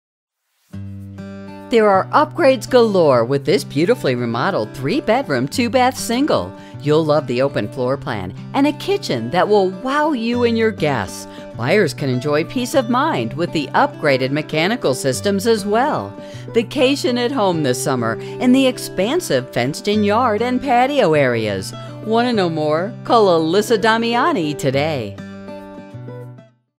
1525 Queen Street Reading, PA | ColdwellBankerHomes.com
1525 Queen Street, Reading, PA

Upgrades galore in this three-bedroom, two-bath Laureldale Single! Pride in Ownership and attention to detail are apparent with this recently remodeled home. You will love the open floorplan boasting 1500 square feet with additional finished square feet in the finished basement featuring a recreatio